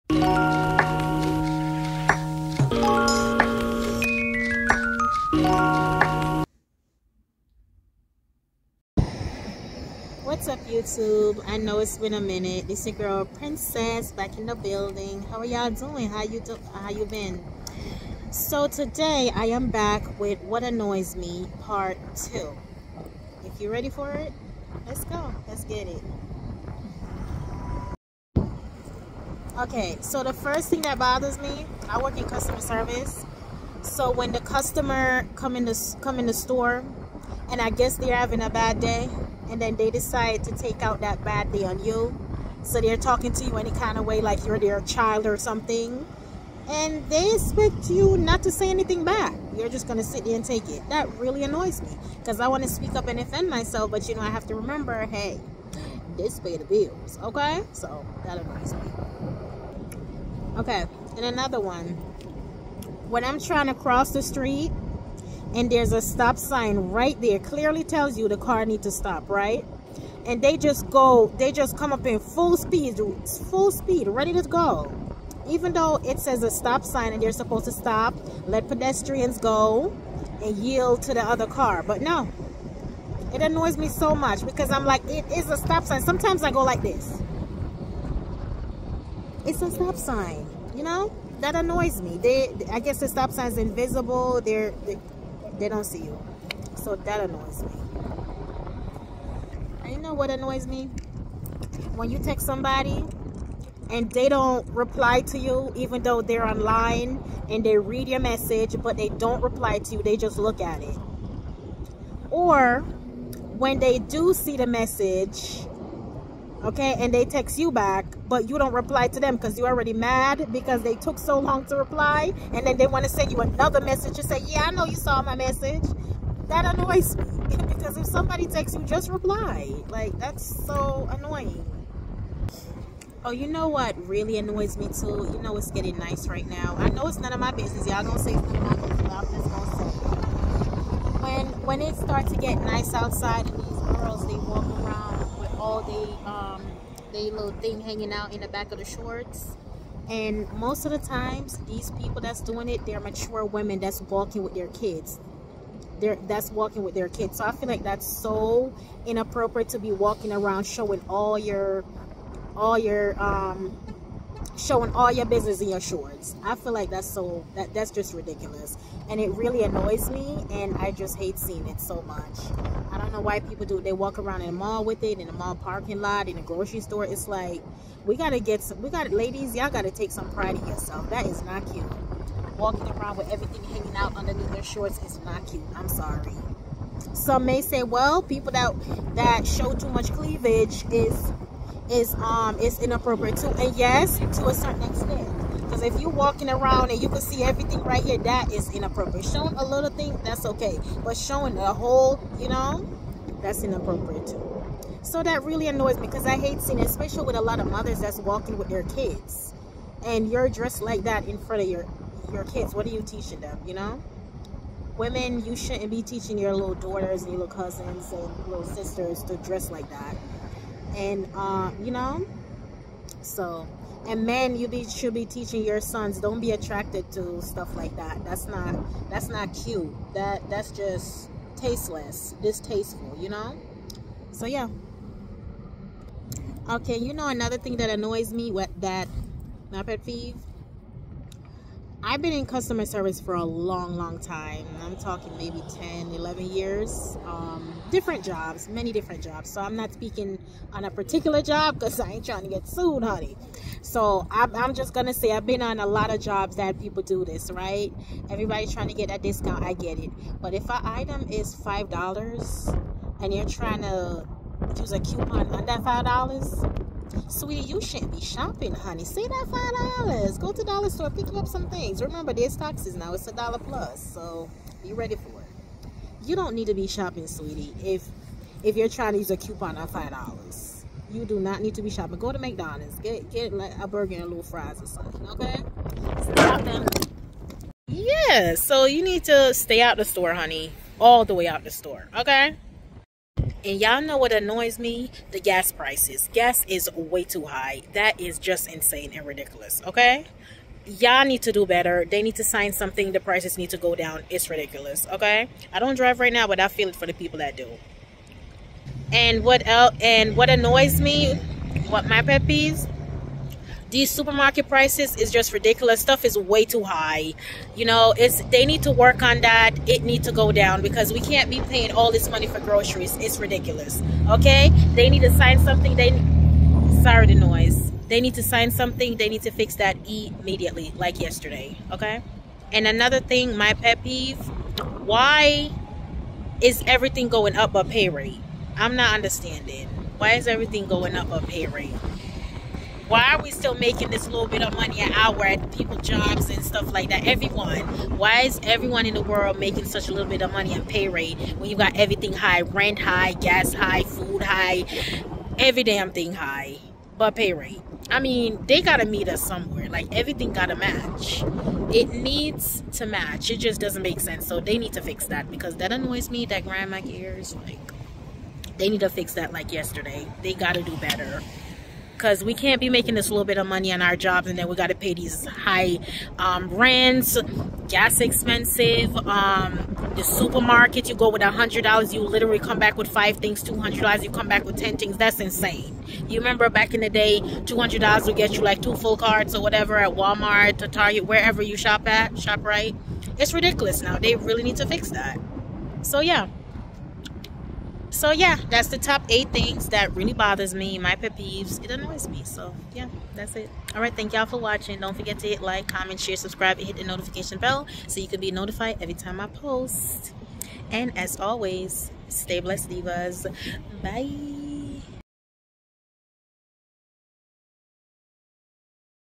8 THINGS THAT ANNOYS ME PART 2
hey Armynation👋🏽
back with a new video for you guys

this is part 2 to the top 10 things that Annoys me my pet Peeves video.